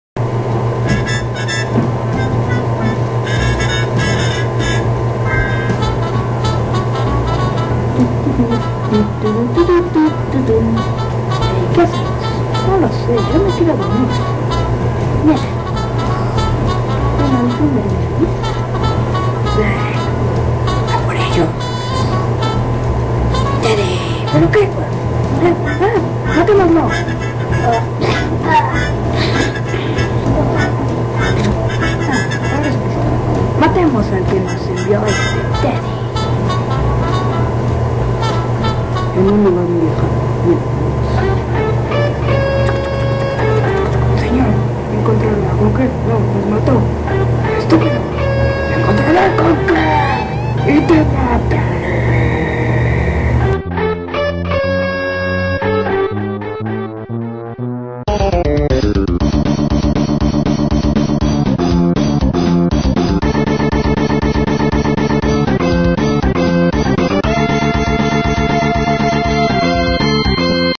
conker and honker Episodio 2 part. 1
Segundo capitulo de Conker y Honker, dos hermanos, y aquí estan sus aventuras.